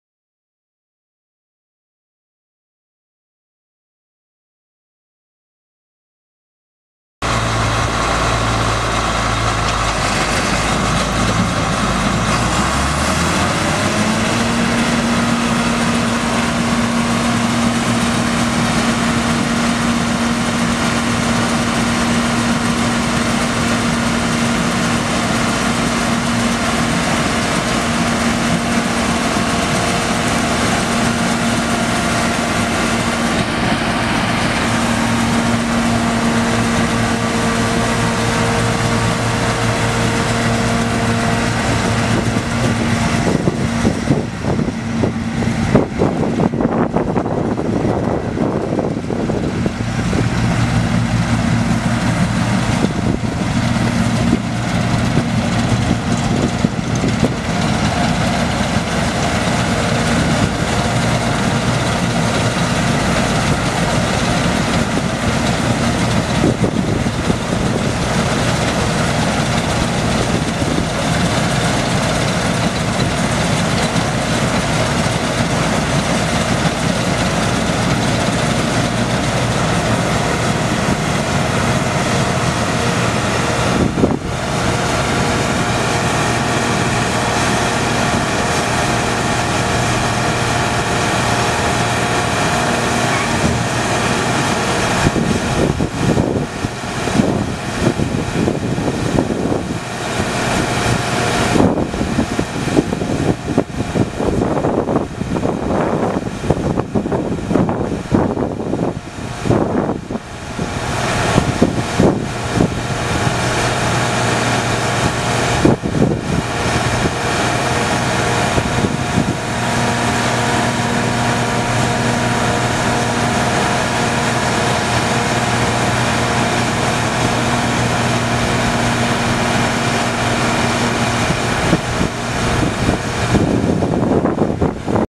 1982 John Deere 6620 Side Hill combine for sale | sold at auction December 16, 2015
1982 John Deere 6620 Side Hill combine
1,546 engine hours on meter
Approx. 3,500 actual hours
John Deere six cylinder turbo diesel engine
Hydrostatic transmission
3F gears
AM/FM
Suspension seat
Chaff spreader
Straw chopper
Two speed
Grain bin
Grain loss monitor
John Deere 216 flex head
16&#039;W
Dual drivelines
Finger reel
Hydraulic reel height control
Steel skid plates
Serial 375778H
28L-26 front tires
11.2-24 rear tires
One owner
Stored inside
Serial 504023

It was located in Leavenworth, KS, and we sold it to the highest bidder for $5,280.